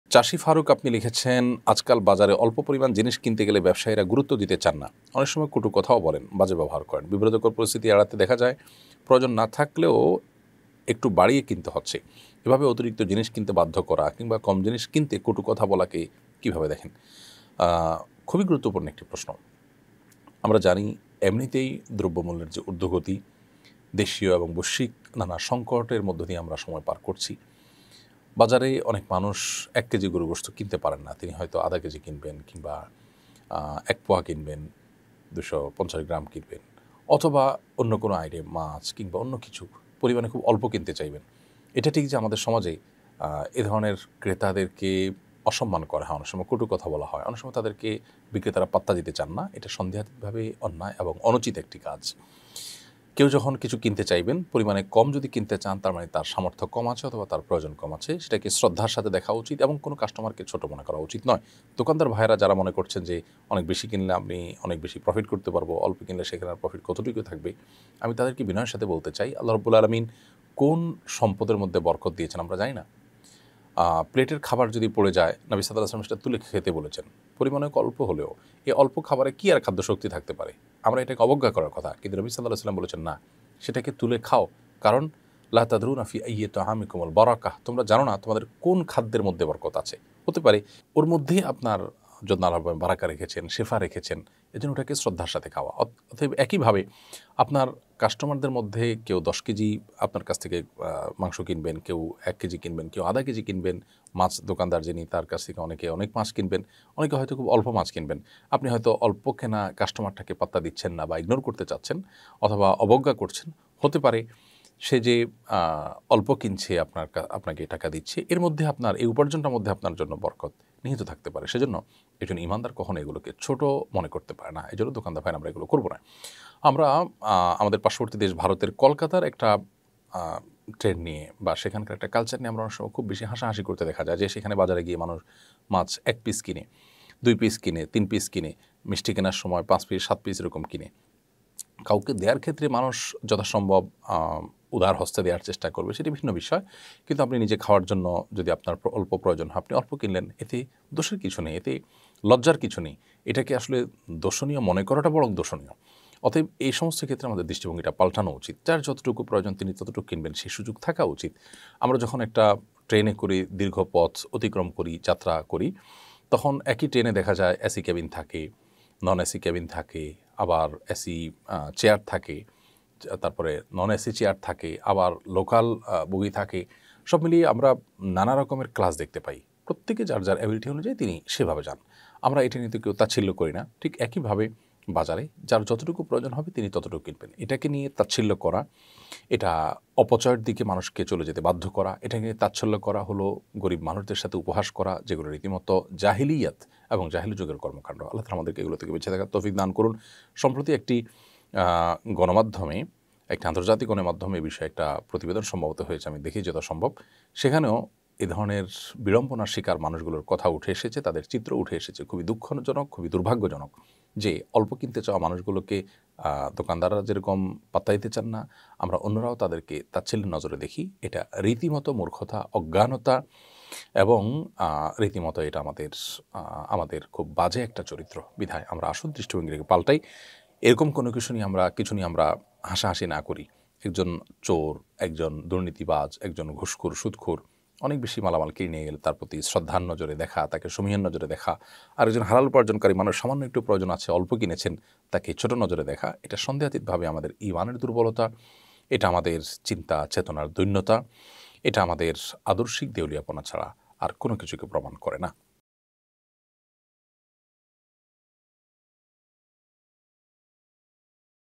অল্প খাবার কিনতে চাওয়া মানুষদের তাচ্ছিল্য বা অবজ্ঞা করা অন্যায়
কলকাতার মানুষের অল্প খাবার কেনা নিয়ে আমরা অনেকে ট্রল করি, দেশেও অনেক দোকানদার কম জিনিস বিক্রি করতে চান না, কম খাবার কিনতে গেলে অপমান করেন, কাজটা যে চরম অমানবিক এবং ত্রুটিপূর্ণ চরিত্র তা অনেকেই জানি না। আসুন, দৃষ্টিভঙ্গিটাকে বদলাই।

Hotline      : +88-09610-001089